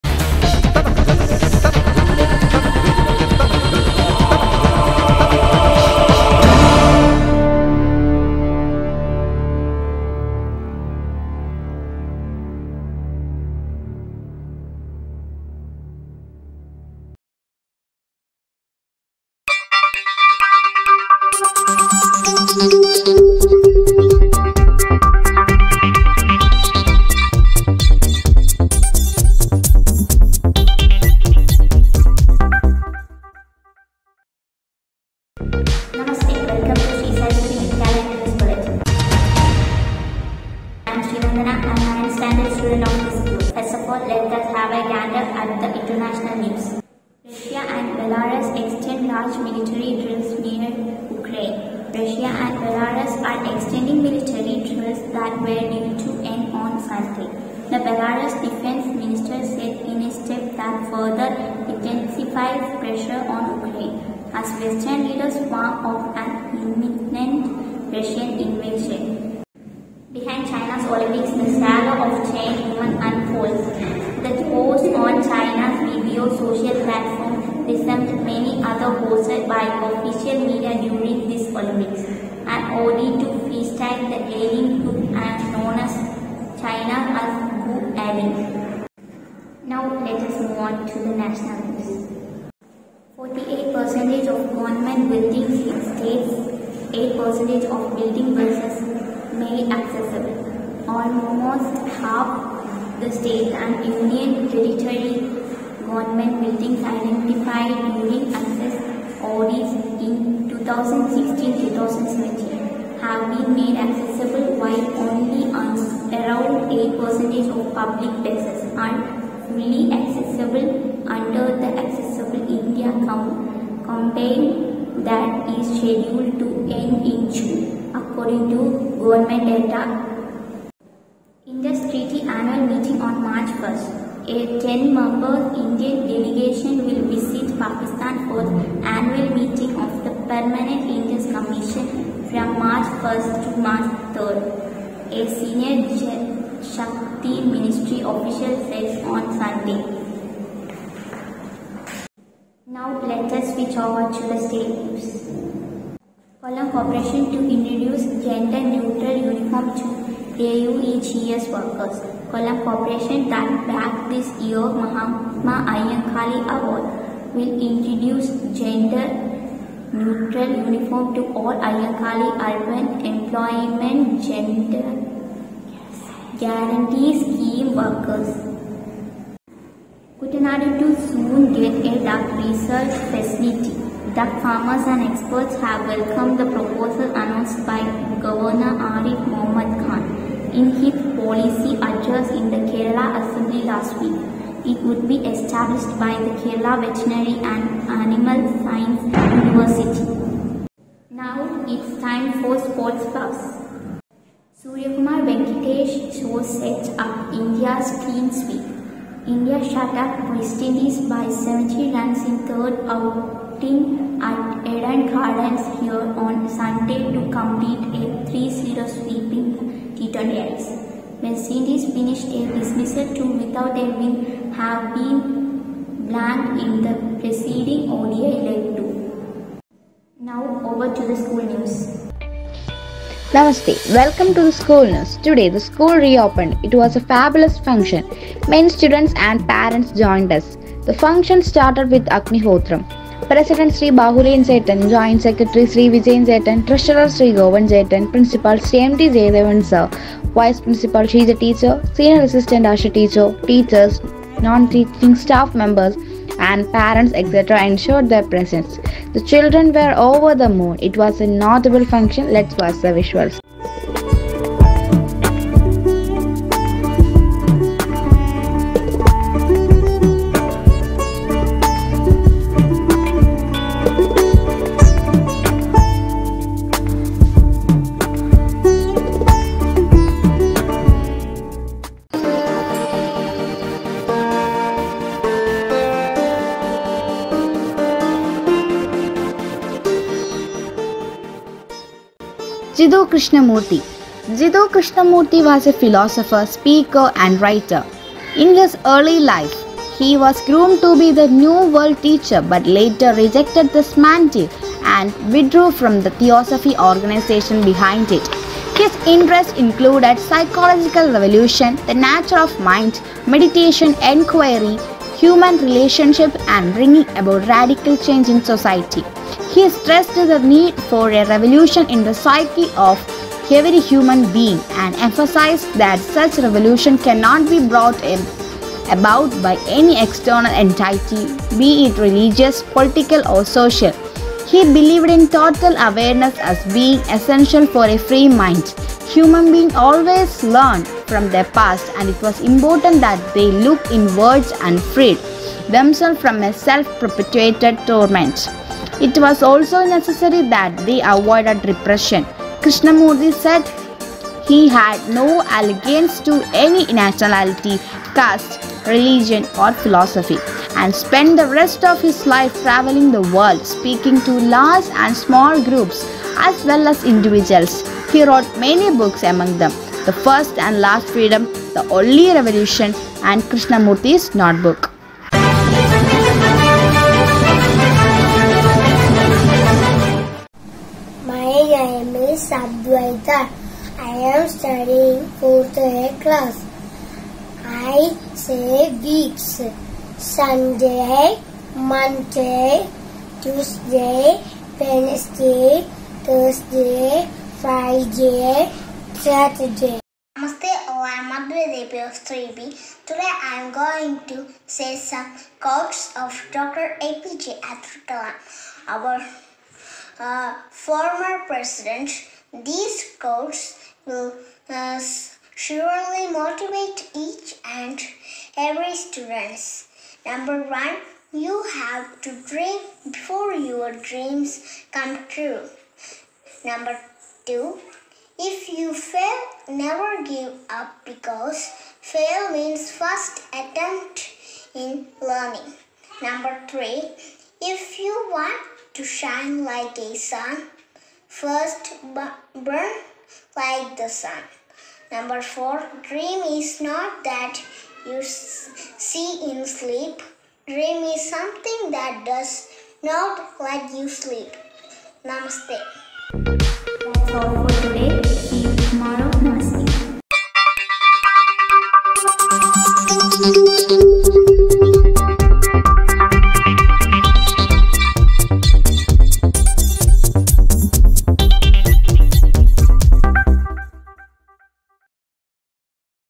SSV NEWS EPISODE 189 22 2 22
Sree Saraswathy Vidyalayam, Ooruttambalam... News Bulletin

   Our child prodigies started a News Bulletin recently. Everyday they read it.
They do not bunk it off. The whole Crew Contains Children. News collection, Editing et.c are done by children themselves. Last week this News appeared in some of the local Newspapers..